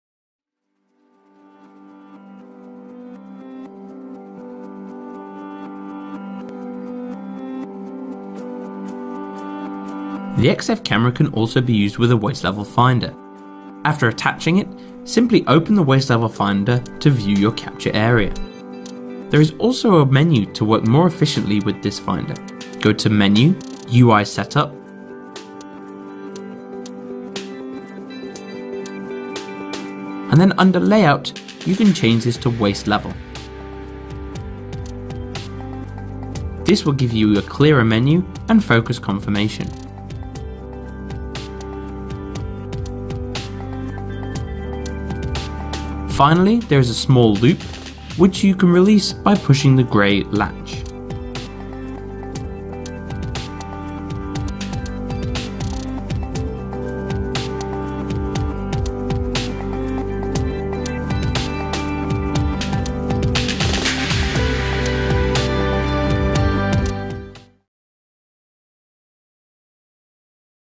[엠아이세븐 | 테크니컬 서포트] 페이즈원 XF 카메라 시스템에서 웨이스트 레벨 파인더 사용하기
이번 비디오에서는 XF 카메라에서 웨이스트 레벨 파인더를 사용하는 방법에 대해 살펴보겠습니다.